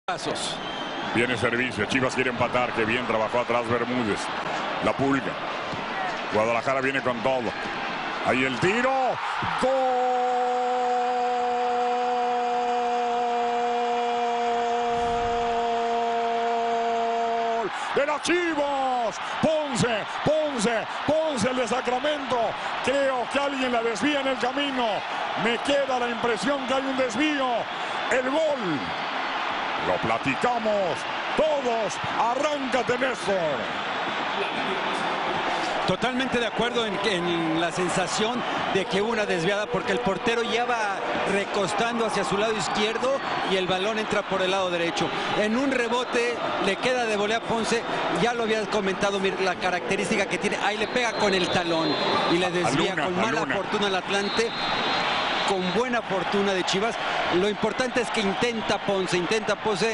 Miguel Ponce anotó sobre la hora y le dio el empate a Chivas
El disparo del defensor rojiblanco se desvió en un rival y venció al guardameta rival.